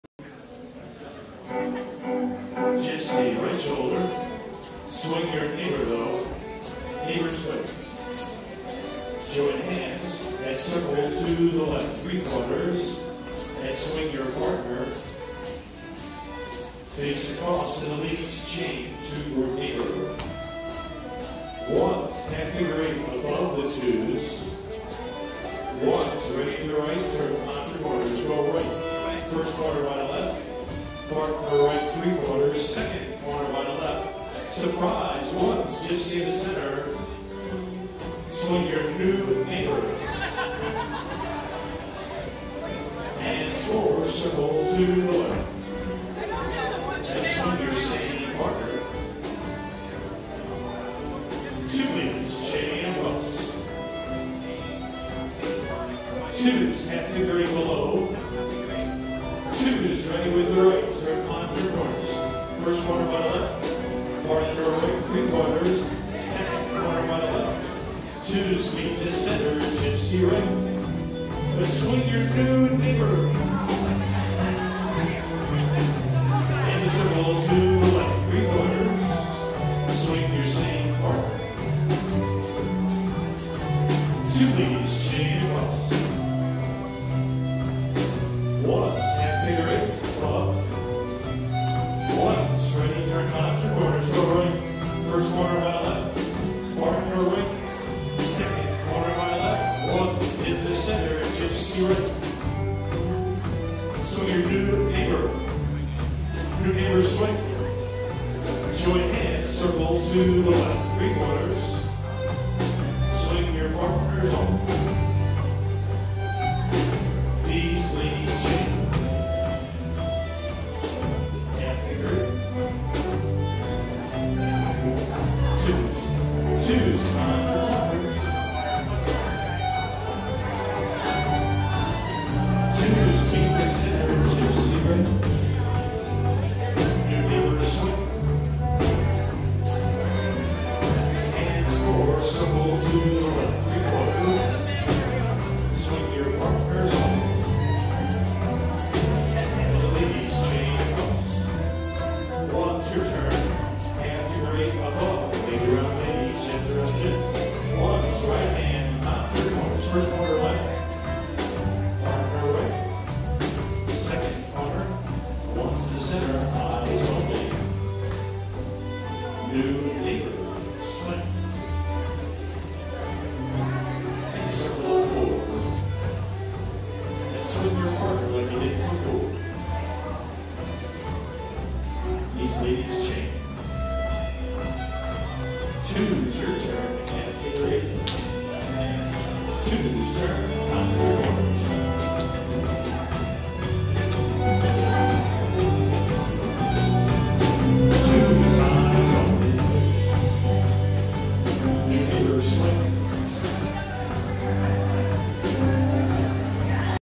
Crystal Vortex 2017
Bob Isaacs calls to tunes by Gaslight Tinkers.
(a brief clip)